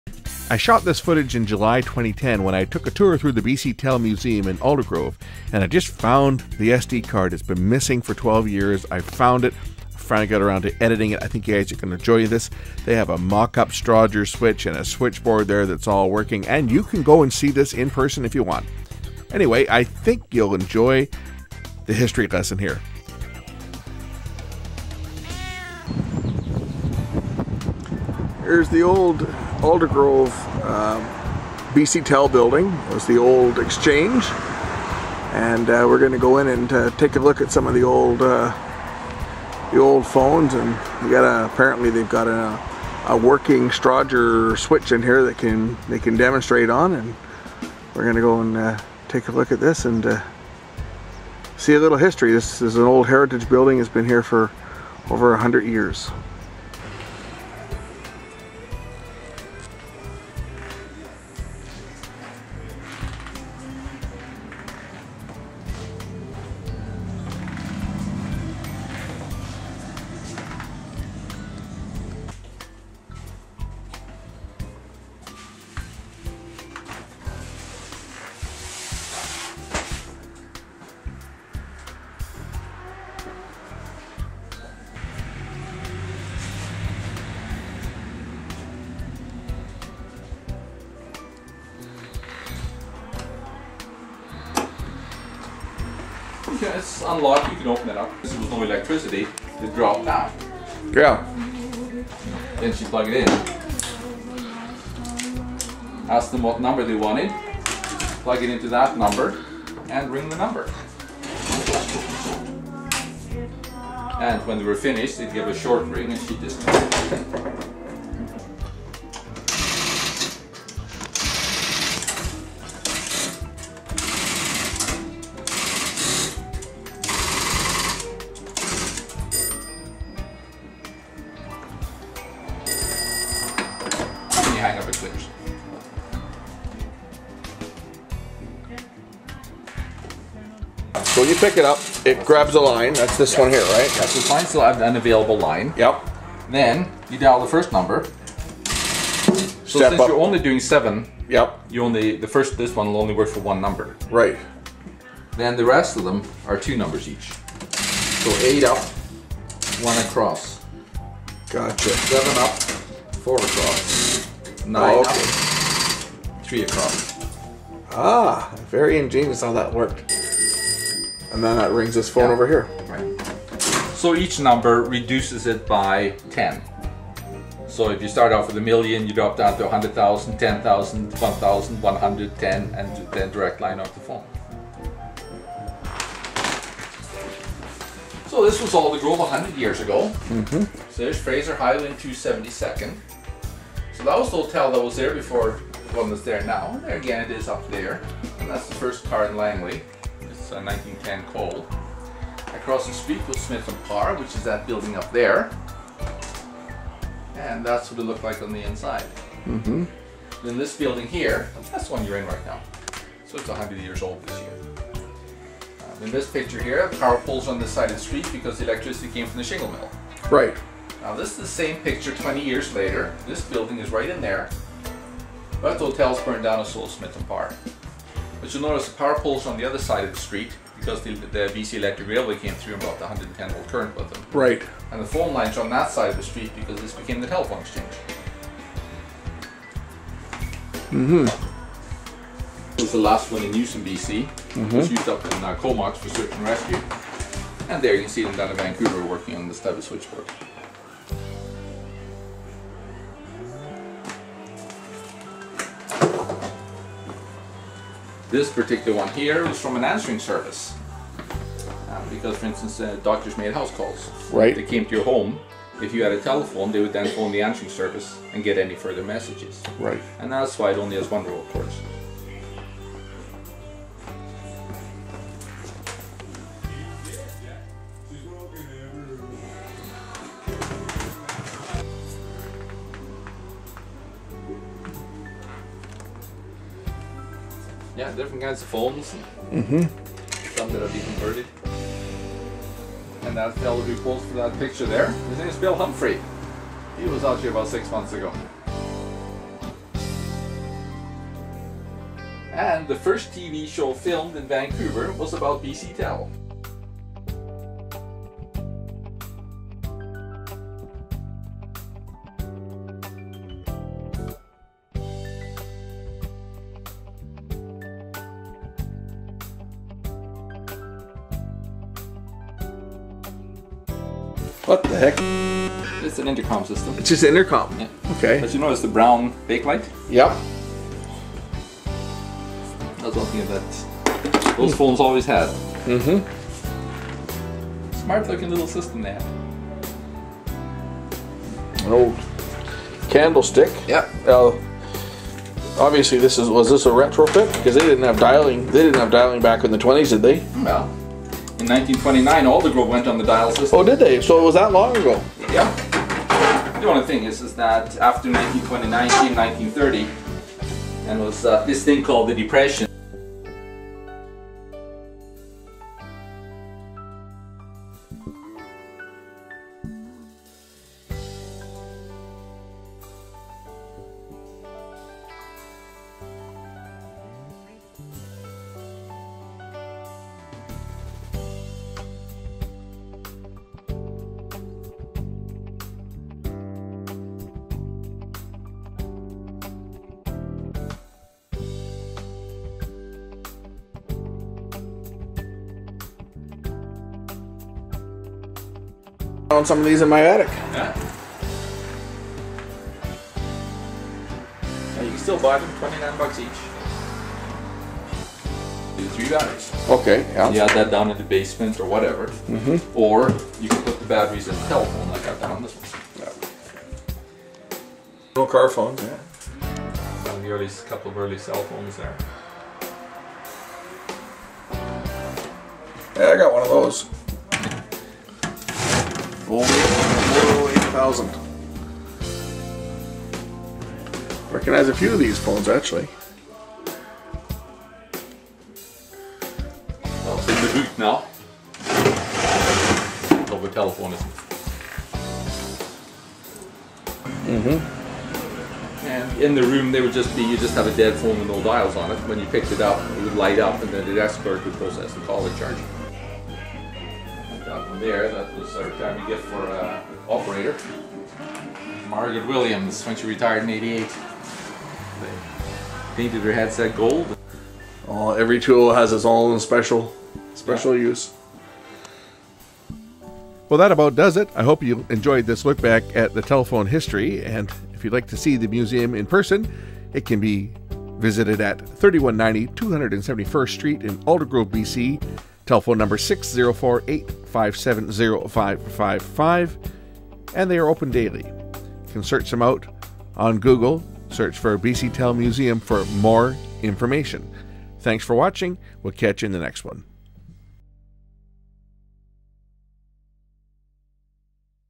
BC Telephone Museum
Original Aldergrove Exchange (heritage building) now museum showcasing the history of the BC telephone company, They have working manual switchboards and a working stroger switch that can dial from one phone to others in the building. Anyone telephone buff will find this interesting and for old telephone workers it will bring back memories.